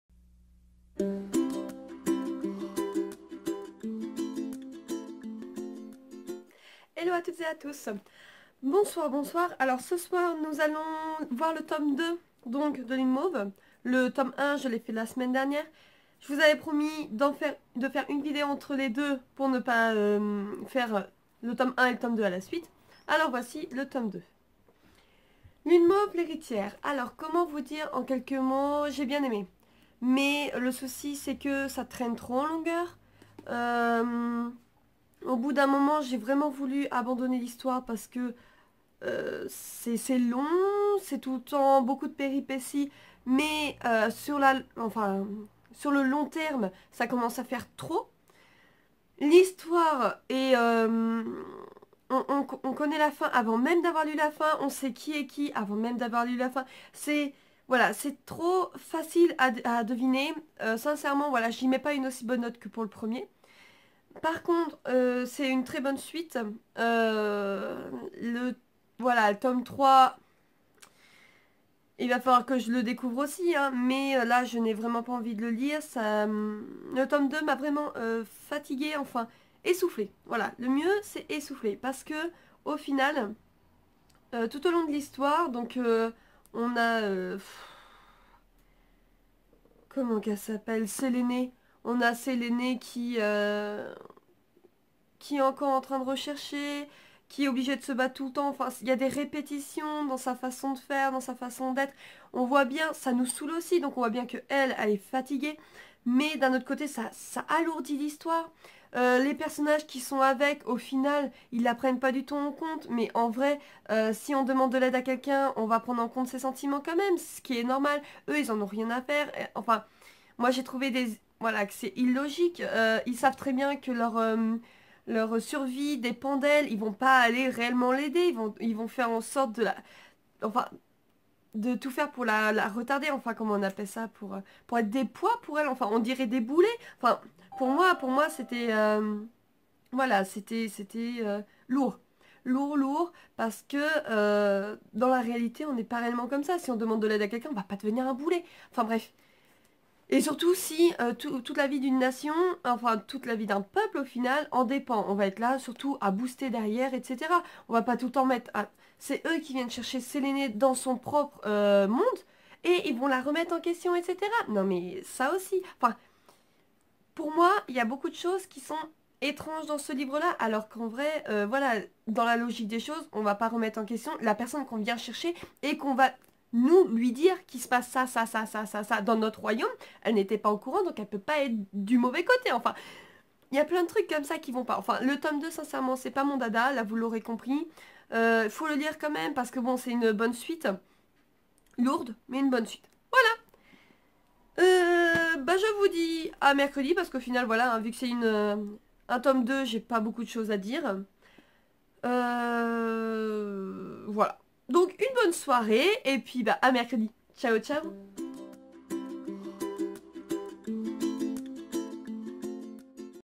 MikiBook : Lune mauve tome 2 l'Héritière de Marilou Aznar
J'ai trouvé ce livre moins bon que le premier, trop de répétitions et lourd au bout d'un moment...
Lisez-le tout de même, ça reste une très bonne suite.